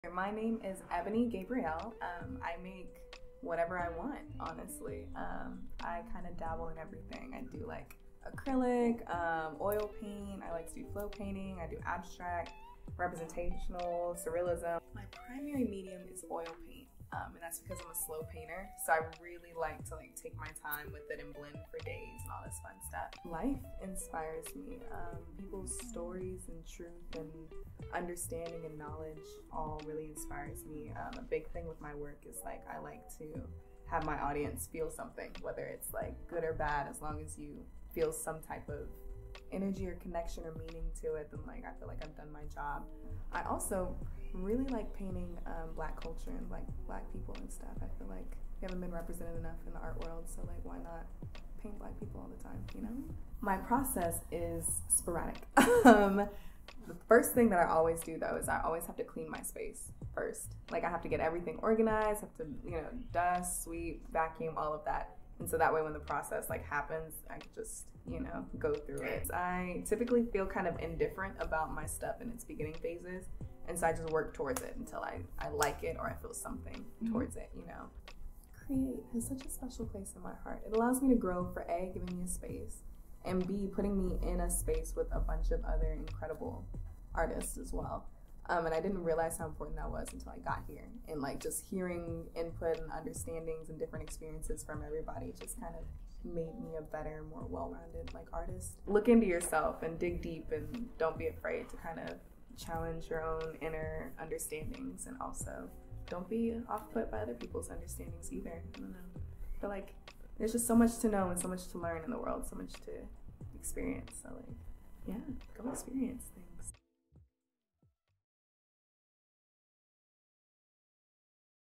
Artist Profile #3 - Ebony Gabrielle
Ebony Gabrielle is a resident artist at Create Arlington. She is a multi-faceted artist and a constant friendly and creative presence in our studios. Get to know her in this artist profile and check out her work!